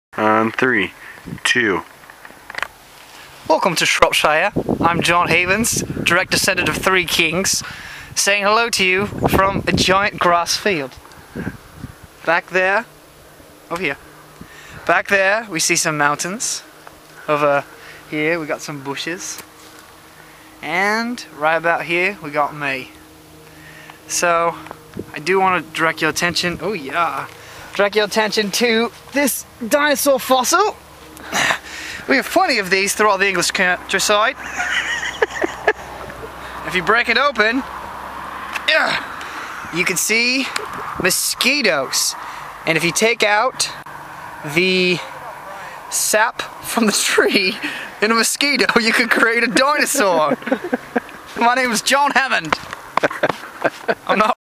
English Countryside
Here we are down the street from our Bed and Breakfast exploring the great greenery that is England